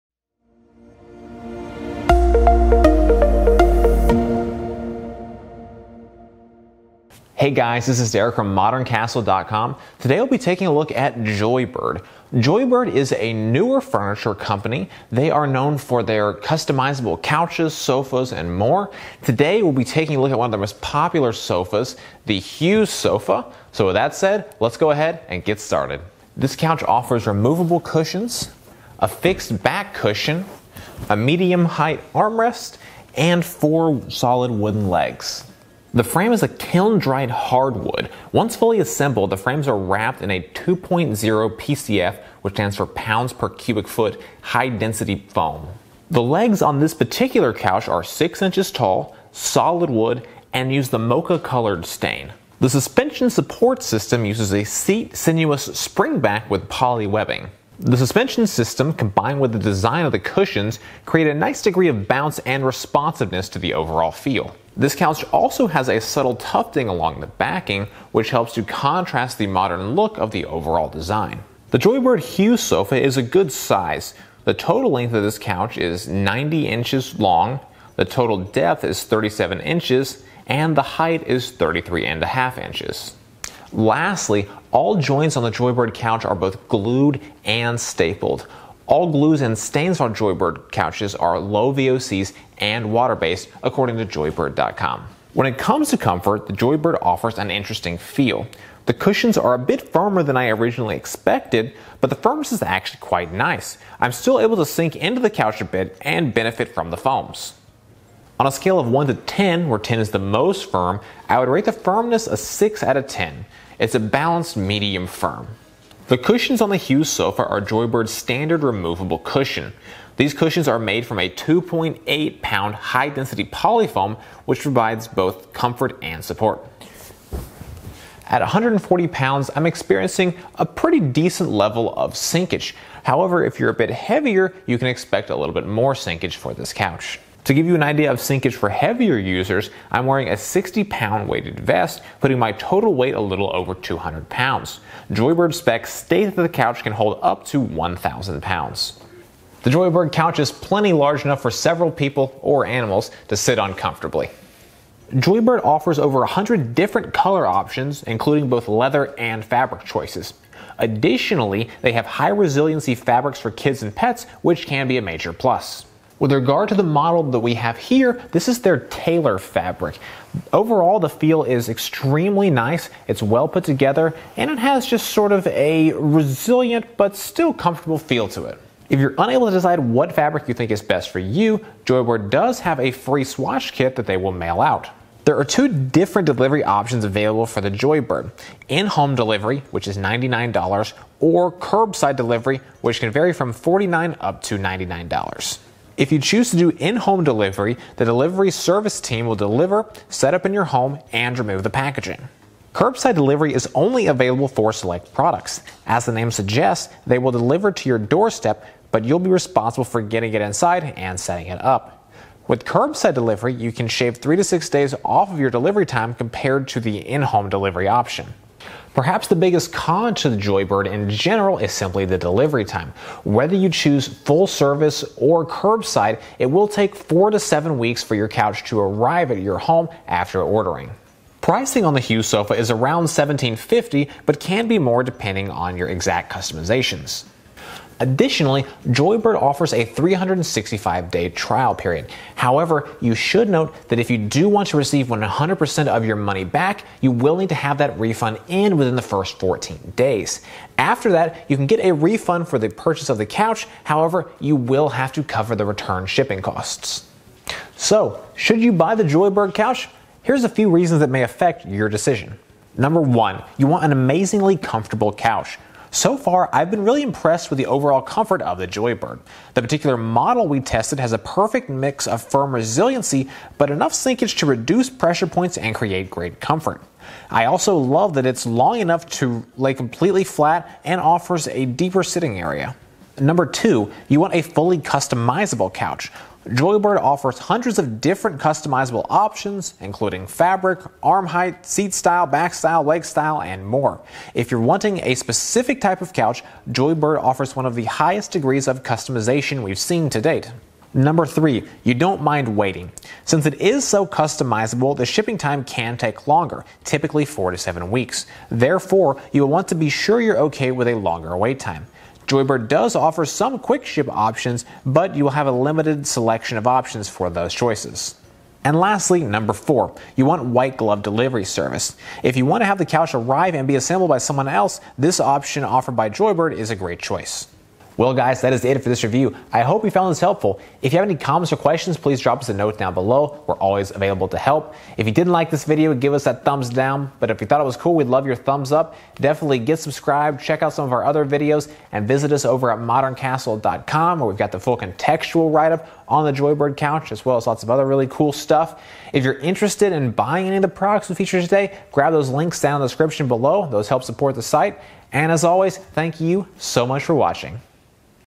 Joybird Sofa Review - Is Joybird the Best Custom Couch?
Joybird is a newer furniture company building one of the most customizable sofas you can find anywhere, but is it comfortable? We answer that question & more in this Joybird review!

We are always happy to help!

We are supported by readers purchasing products we feature. Questions? See how Modern Castle works here: